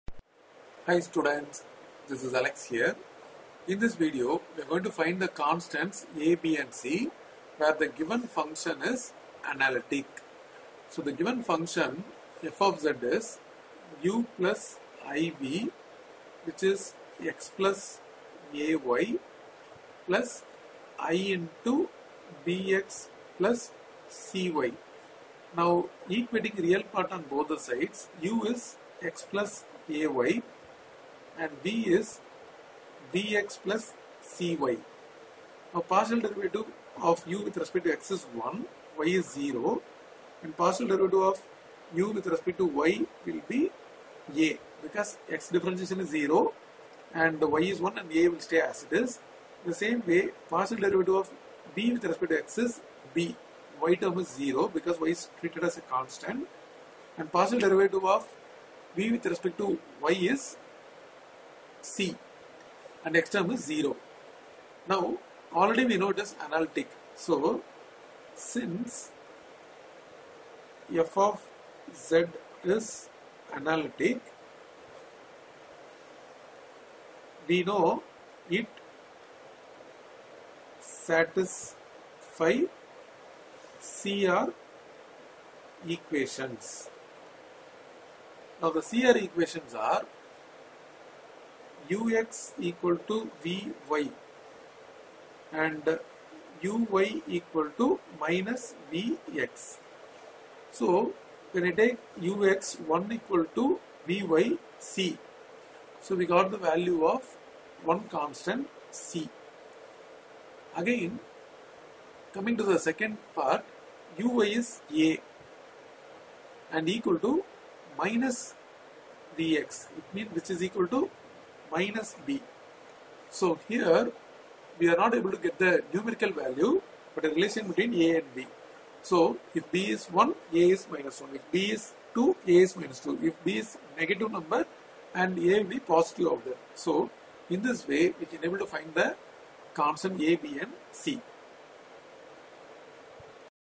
CR Equations Problem 9 | Cauchy – Riemann Equations |Analytic Functions | Engineering Maths
Find the constants a, b and c if
f(z)=(x+ay)+i(bx+cy) is analytic